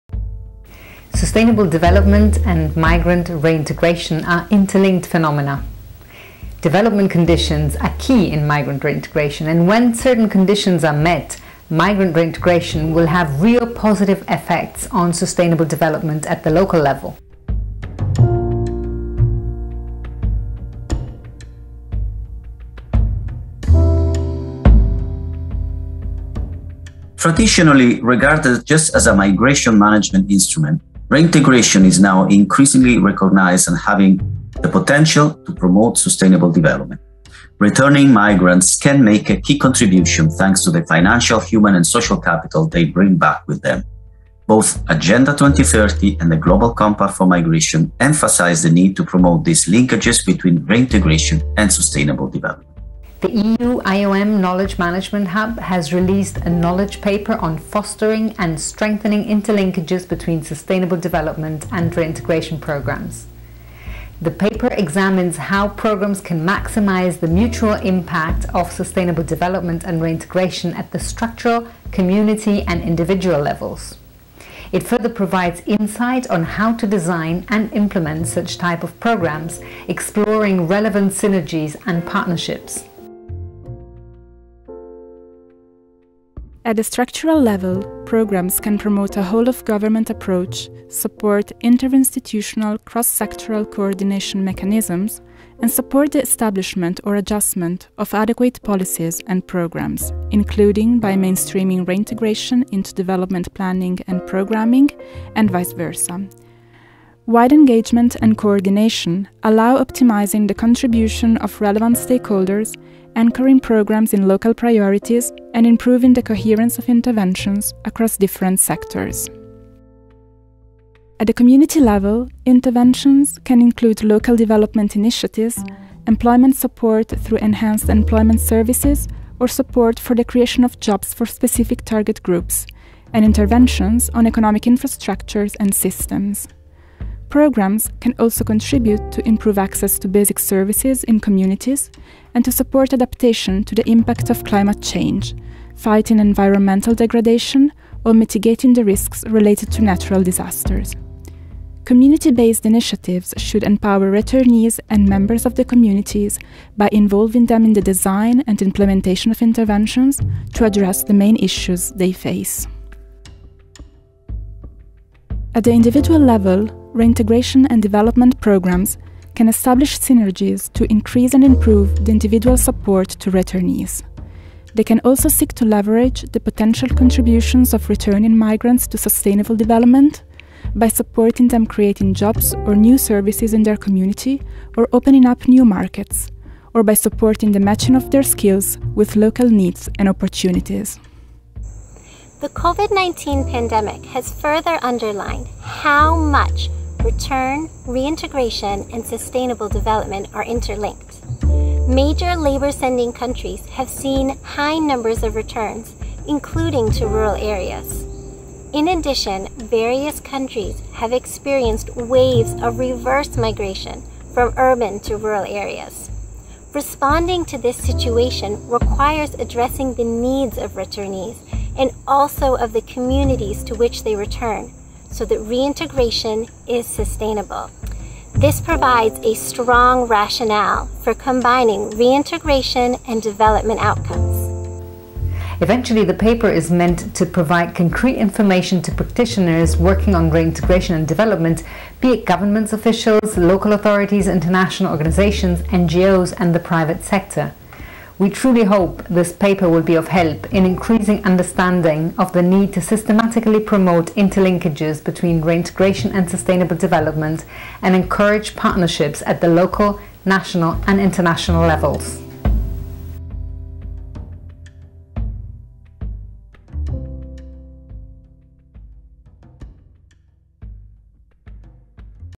Migrant reintegration and sustainable development: Fostering and strengthening interlinkages
The EU-IOM Knowledge Management Hub (KMH) released the second issue of the Sustainable Reintegration Knowledge Paper Series focusing on the interlinkages between sustainable development and reintegration.

Presenting case studies from IOM, FAO and other UN Agencies, GIZ and governments, the paper examines how programmes can maximize the potential positive mutual impact of sustainable development and reintegration through interventions at the individual, community and structural level, and how programmes can be designed and partnerships established to pursue sustainable reintegration and sustainable development.